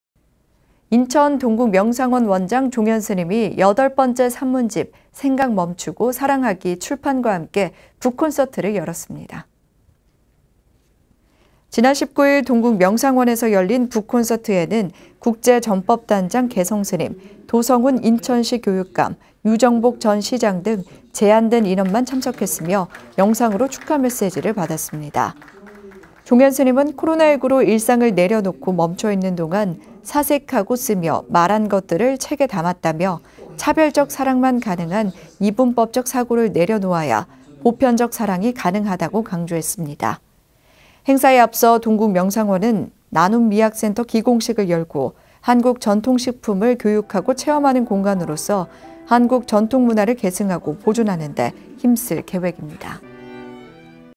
[BTN뉴스] 동국명상원 원장 종연스님 산문집 북 콘서트
인천 동국명상원 원장 종연스님이 8번째 산문집  ‘생각 멈추고 사랑하기’ 출판과 함께 북 콘서트를 열었습니다.

지난 19일 동국명상원에서 열린 북콘서트에는 국제전법단장 계성스님, 도성훈 인천시교육감, 유정복 전 시장 등 제한된 인원만 참석했으며 영상으로 축하 메시지를 받았습니다.

종연스님은 코로나19로 일상을 내려놓고 멈춰있는 동안 사색하고 쓰며 말한 것들을 책에 담았다며 차별적 사랑만 가능한 이분법적 사고를 내려놓아야 보편적 사랑이 가능하다고 강조했습니다.

행사에 앞서 동국명상원은 ‘나눔미학센터’ 기공식을 열고, 한국전통 식품을 교육하고 체험하는 공간으로써 한국전통문화를 계승하고 보존하는데 힘쓸 계획입니다.